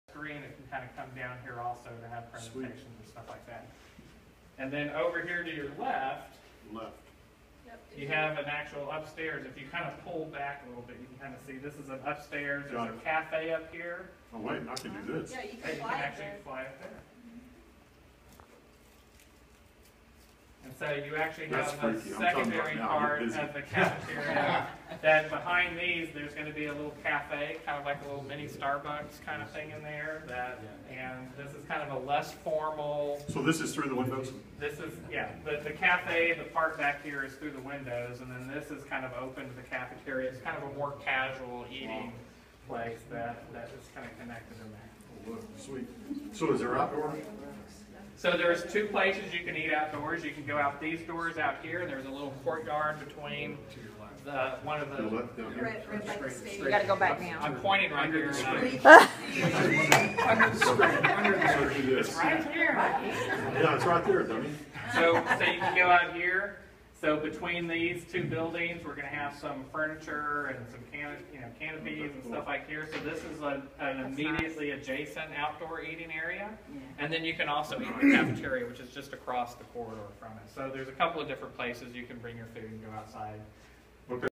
CISD board experiences virtual reality
The Cleburne ISD board of trustees participated in a virtual reality experience of artist renderings of the new Cleburne High School during a special called work session on Monday night.
Corgan Architects Vice President and Project Manager Doug Koehne directs CISD Board Vice President John Finnell where to go in different areas of the school.